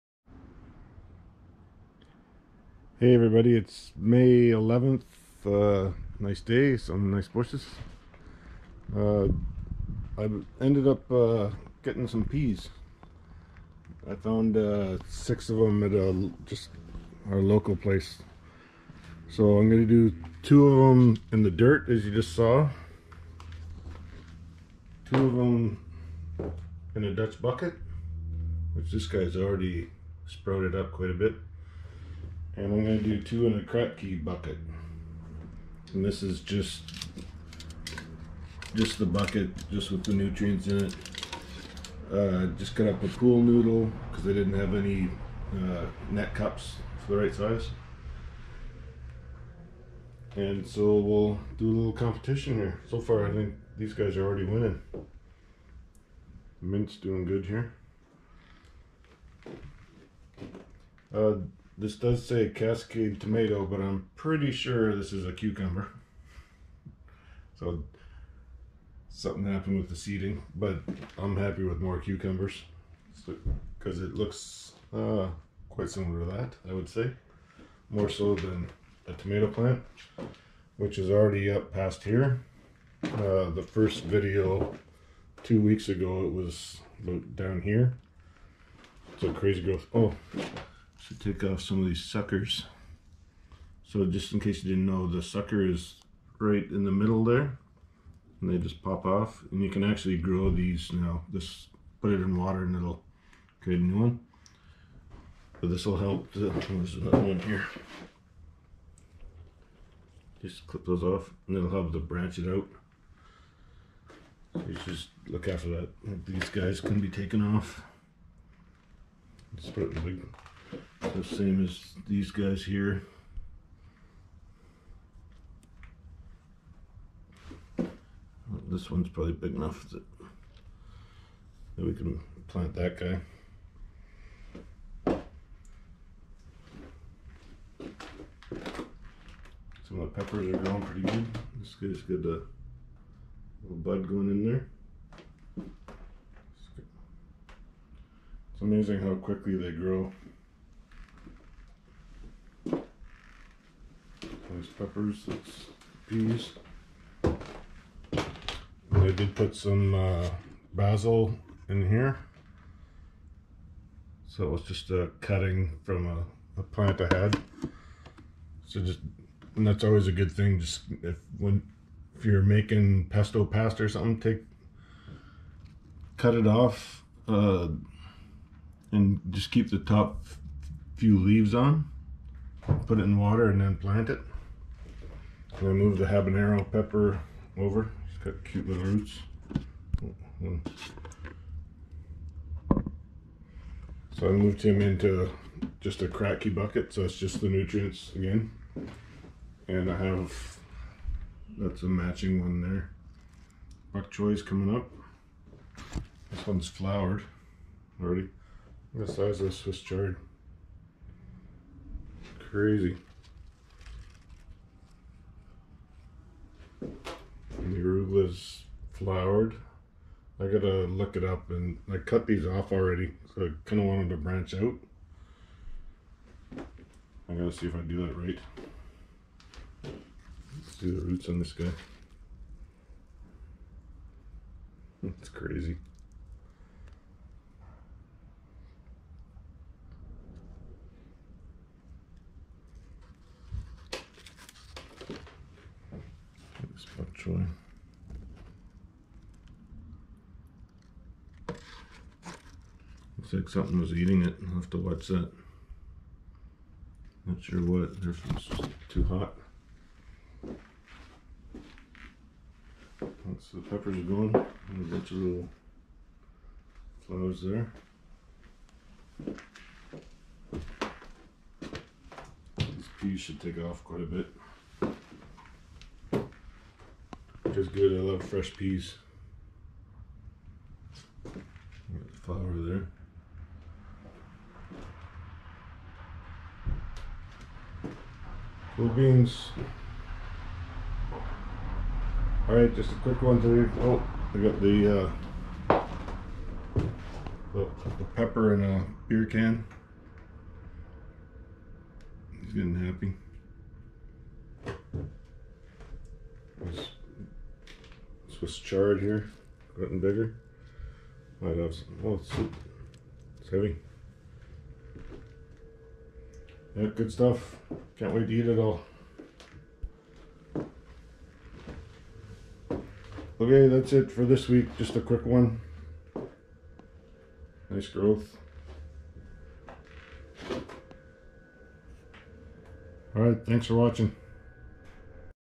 My Hydroponic Greenhouse, 3rd week of growth
Third week of growth for this year in my hydronic greenhouse using 15 Dutch Buckets, 3 NFT rails, a couple kratky style buckets (so far) and a Pepper In A Beer Can.

Documenting the weekly growth so I can see what works and what doesn't.

Lots of peppers, tomatoes, cucumbers, arugula and swiss chard currently. Finally got some peas, doing a comparison between soil, kratky and dutch bucket to see which grows best. Just put them in yesterday, and the dutch bucket ones seem to have taken off already.

Any tips on improving my set up are always helpful. I need to see about the arugula and bok choy flowering already...